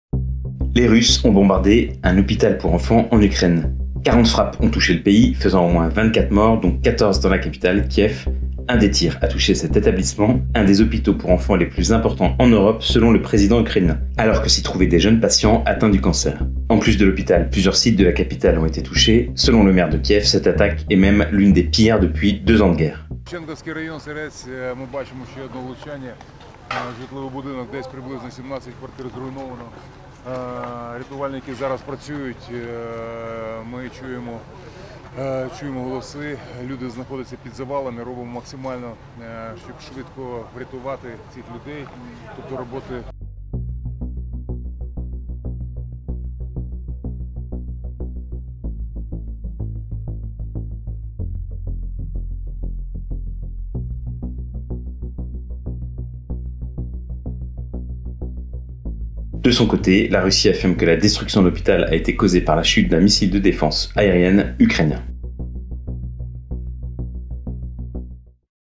« Plus de 40 missiles » tirés en Ukraine, un hôpital pour enfants malades détruit
Les Russes ont mené une attaque massive sur l'Ukraine. Un premier bilan fait état de dizaines de morts et de nombreux blessés. Tandis qu'un hôpital pour enfants, l'un des plus grands d'Europe selon le président ukrainien a été en partie détruit par un tir.